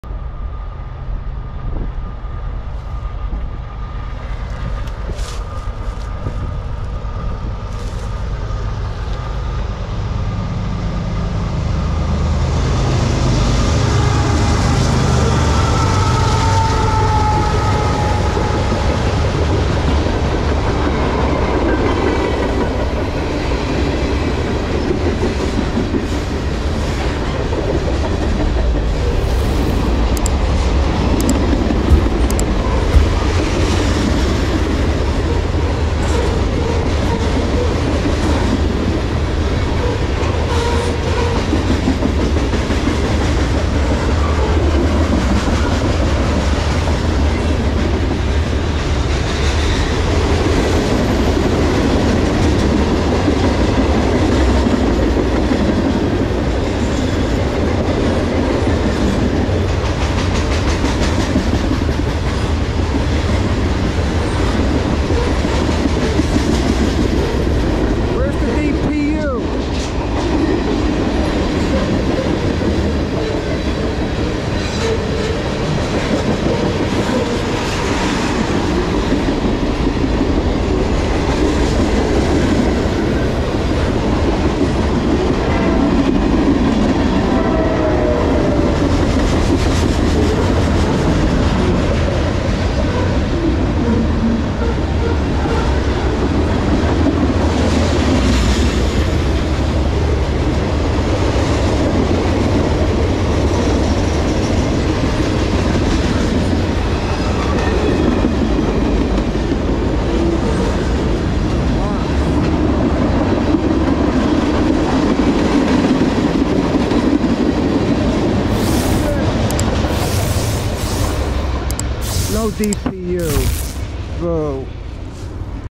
01/30/2023 CSX Lincoln Log Train- MIddlesex, NJ- CSX #5415 / #3285 - 384 Axles, 36 MPH
CSX 5415 (GE ET40DC) leads 3285 (GE ET44AH) with 384 Axles going 36 MPH with a mixed manifest load with a lot of train cars filled with lincoln logs!  Who remembers them? LOL
Please checkout all my other videos in all my train playlists.  There's something for everyone!
ALL are greatly appreciated.
I thank you all in advance :-)